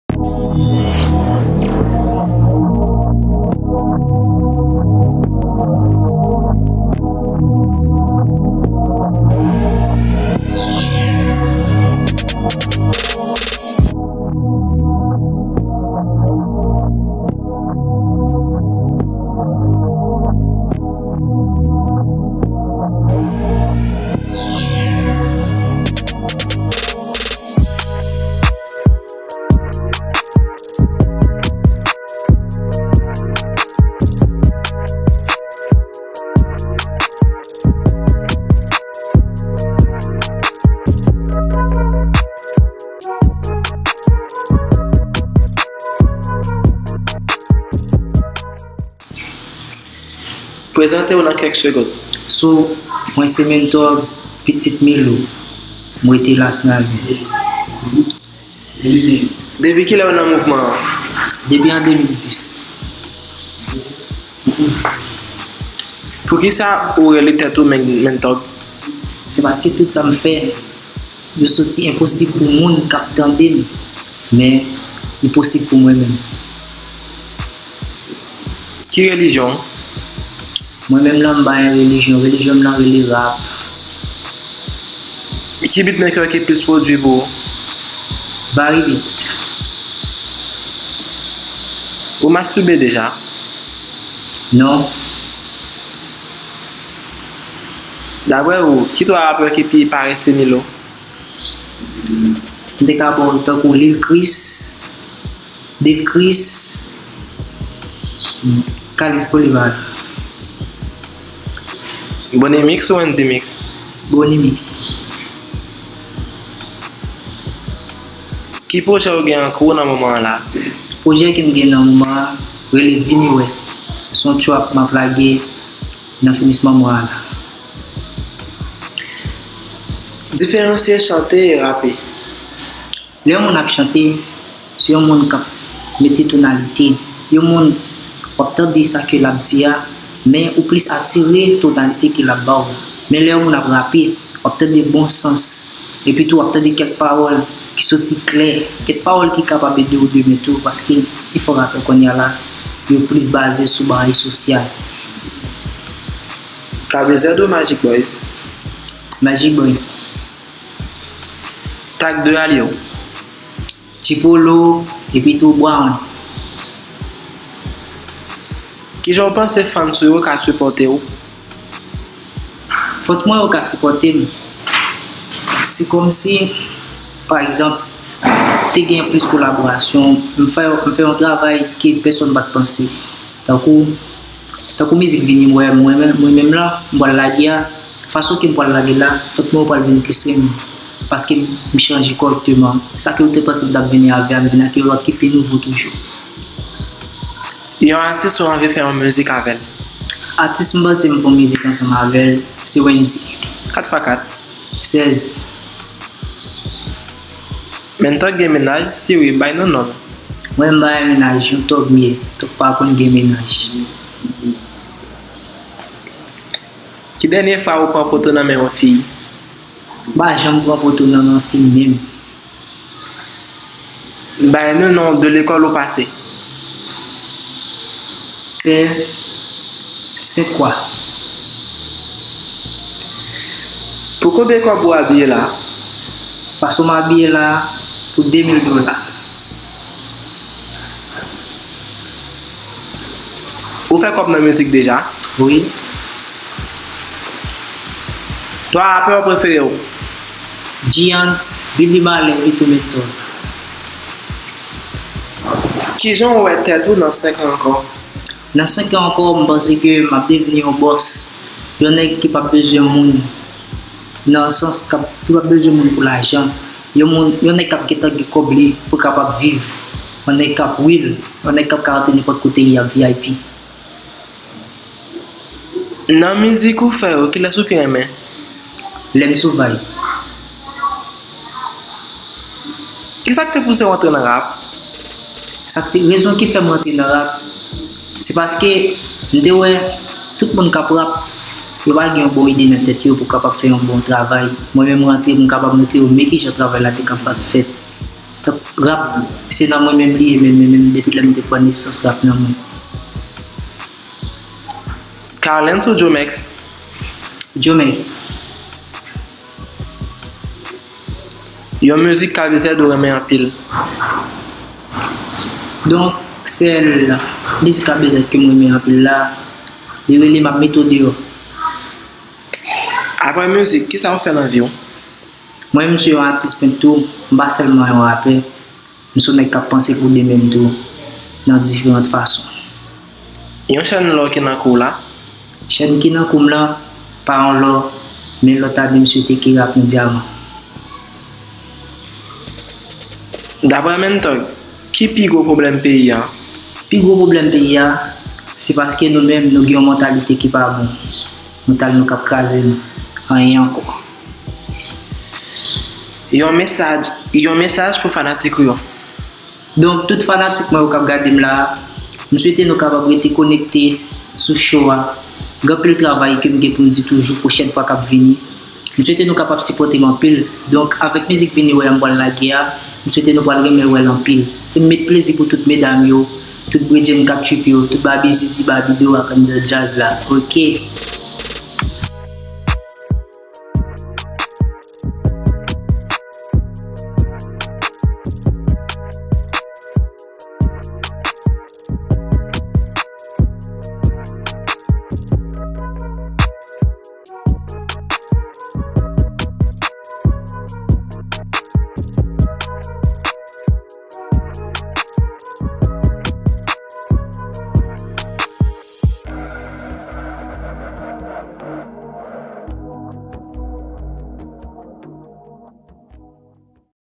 NEW CHRISTO SHOW AVEK BLAISE JHONSLY (MENTHUG)
CHRISTO TECH POTE POU OU NOUVO CHRISTO SHOW LI HAN AVÈK MENTHUG YON JEUNE ATIS KI RETE MILO.. VIN TANDE SA ATYS LA DI... PA BLIYE ABONE,PATAGE LIKE